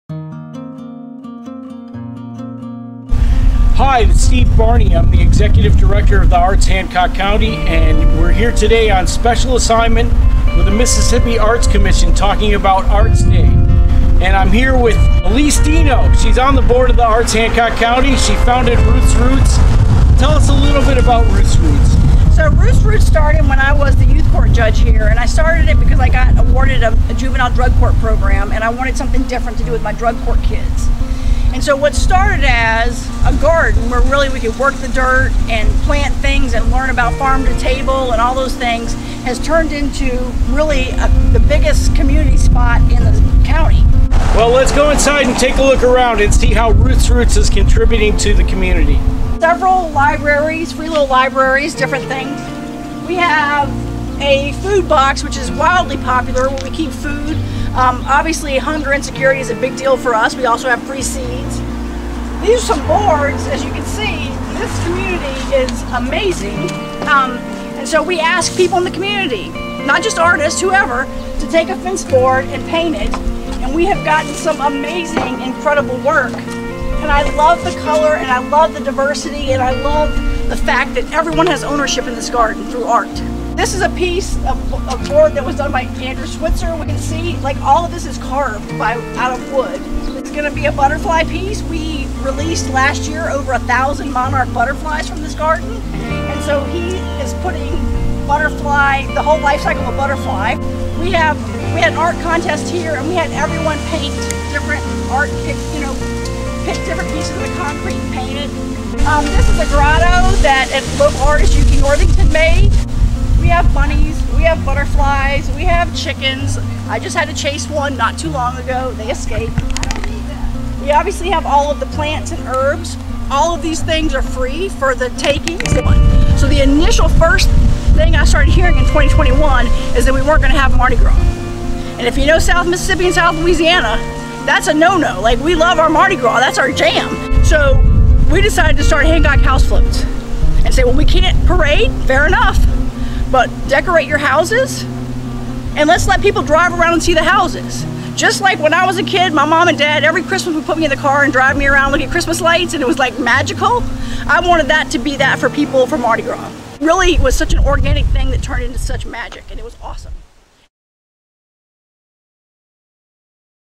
Mississippi Arts Day 2021 Bay St. Louis
March 25, 2021 In celebration of Arts Day 2021, The Arts Hancock County examines how the arts our making an impact in our community. We spoke with Elise Deano to learn about Ruth's Roots and Hancock House Floats.